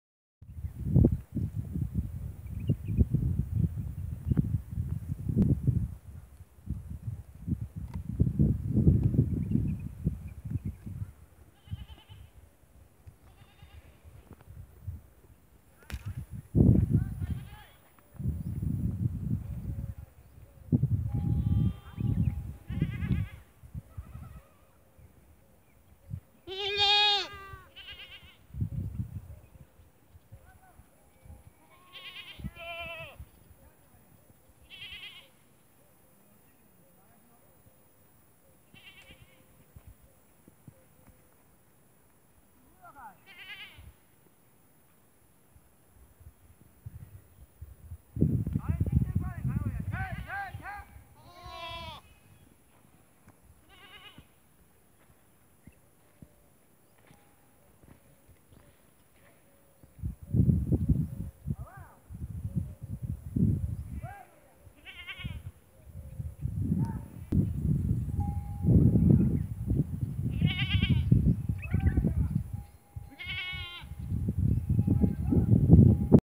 Goats Farming in Desert Thar
Goats in desert tharparkar sindh Pakistan

goats farming

goats videos